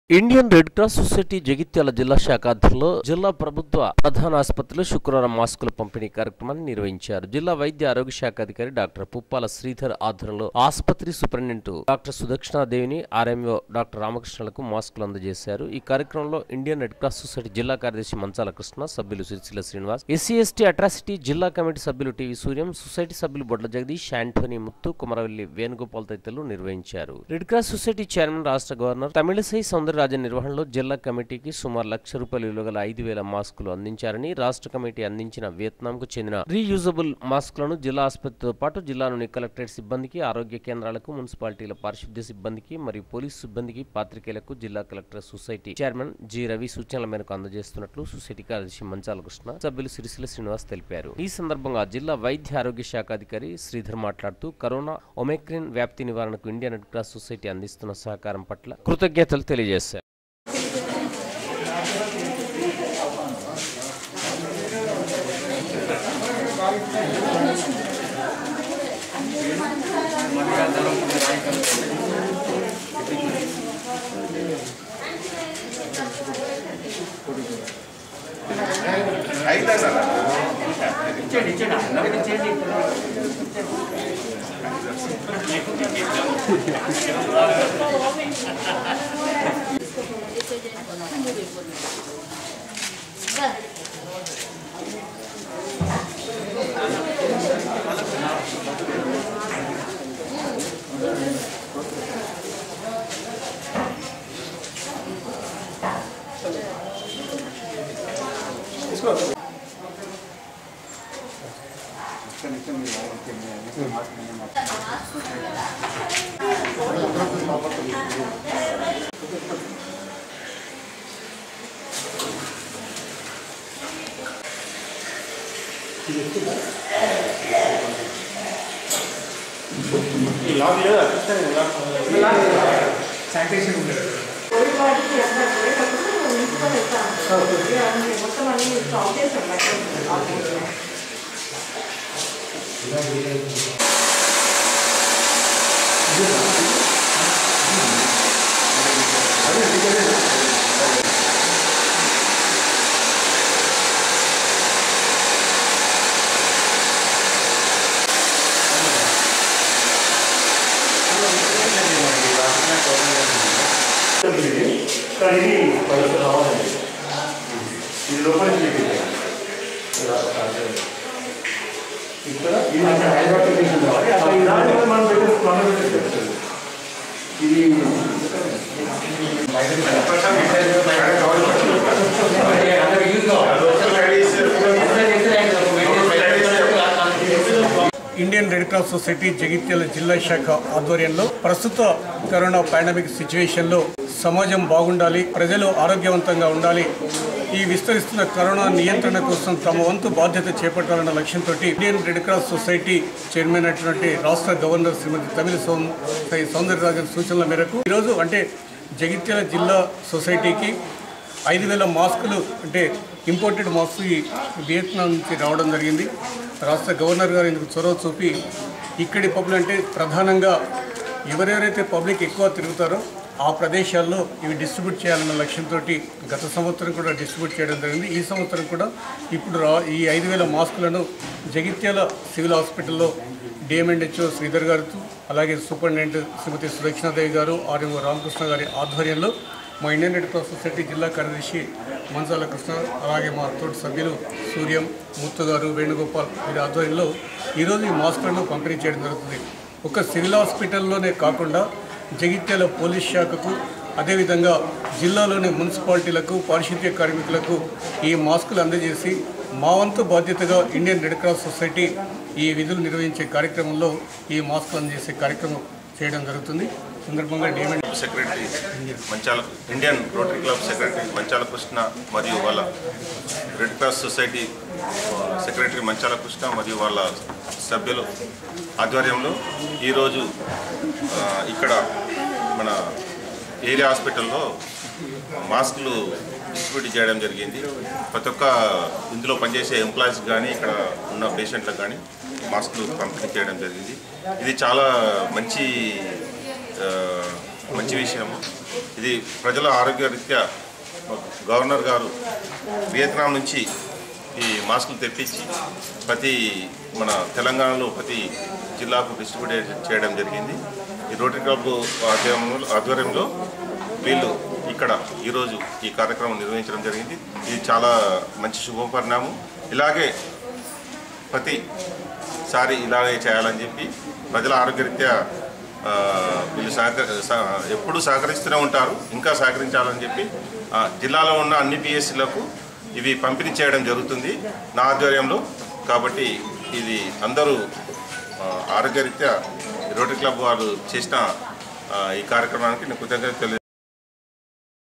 14.1.22| DSC | Jagtial |రెడ్ క్రాస్ ఆధ్వర్యంలో ఏరియా ఆసుపత్రిలో మస్కులు పంపిణీ
| Durga Siti Cable | News |రెడ్ క్రాస్ ఆధ్వర్యంలో ఏరియా ఆసుపత్రిలో మస్కులు పంపిణీ